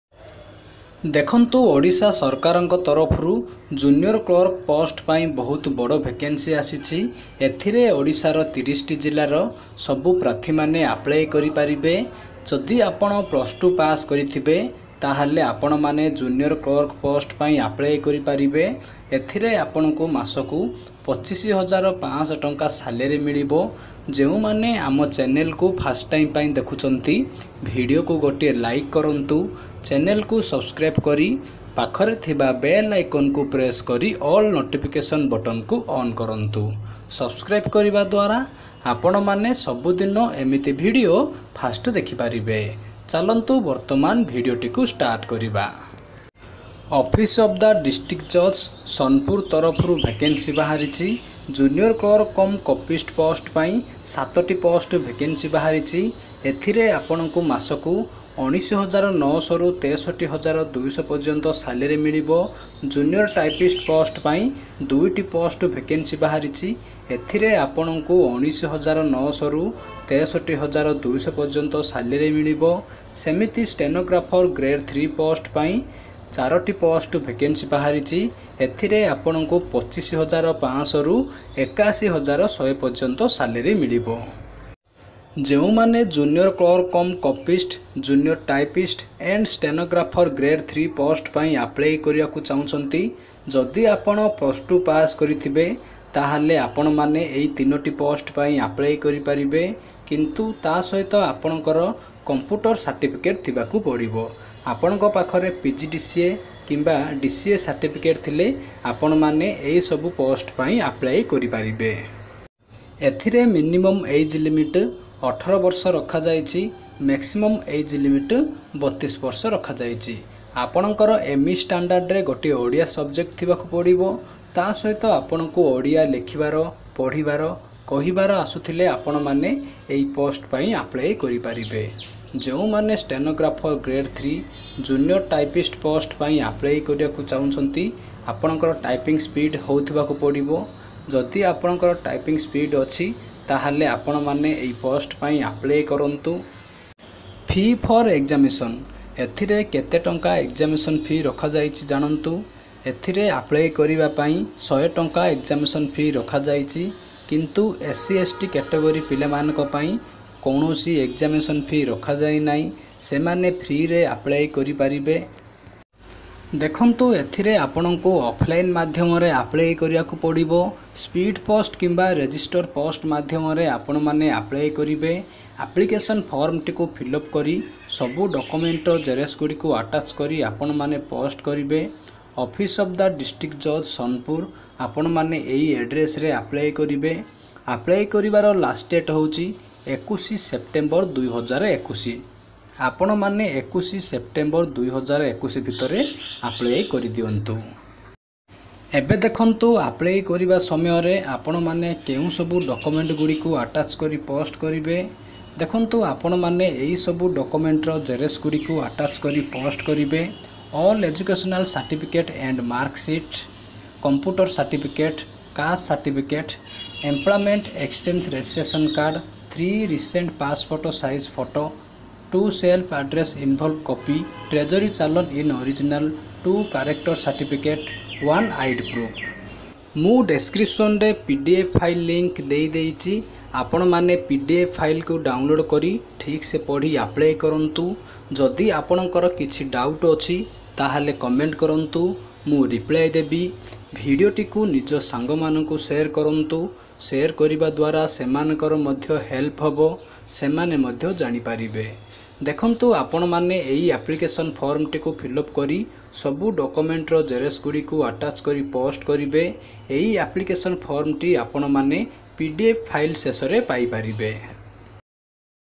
Odisha Junior Clerk Vacancy 2021. Salary:- 25,500. Odisha New Govt. Job 2021.
Odisha Junior Clerk Recruitment 2021.

Odisha junior Typist recruitment 2021.

Odisha Stenographer recruitment 2021.

Odisha district level job 2021.

MAHESWAR SETHI